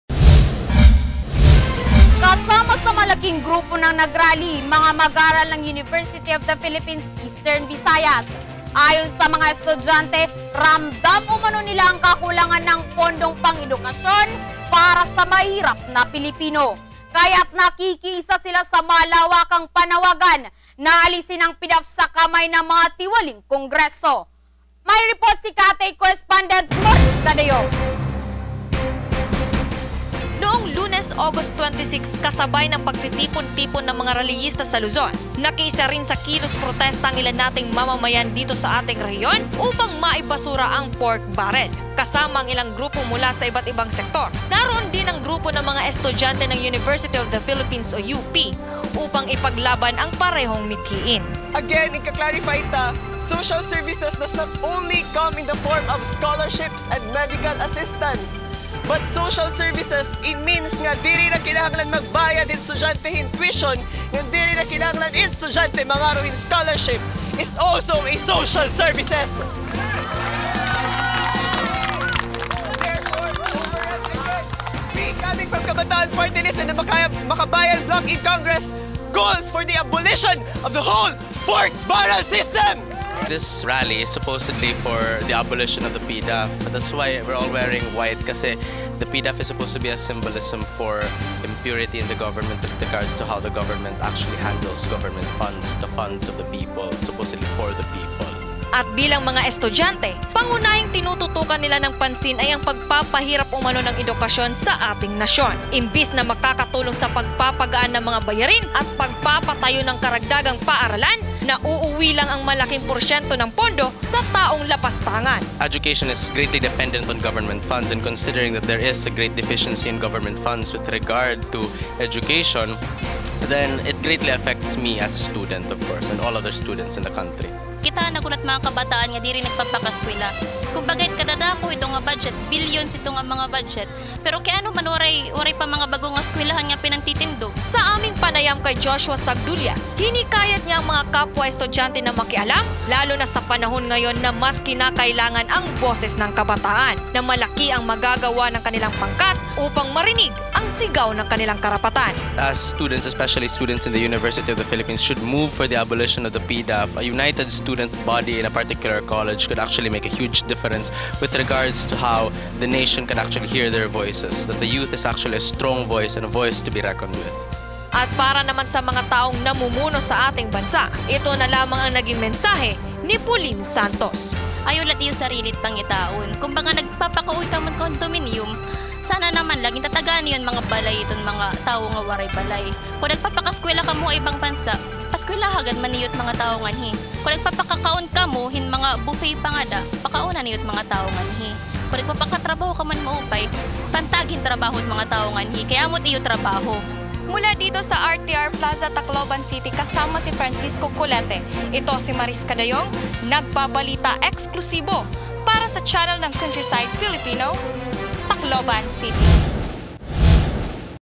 Dateline Tacloban: Mga Esudyante Nakiisa Upang ang Pork Barrel System Mabasura!
Dateline Tacloban: August 28, 2013:  Mga Esudyante Nakiisa Upang ang Pork Barrel System Mabasura!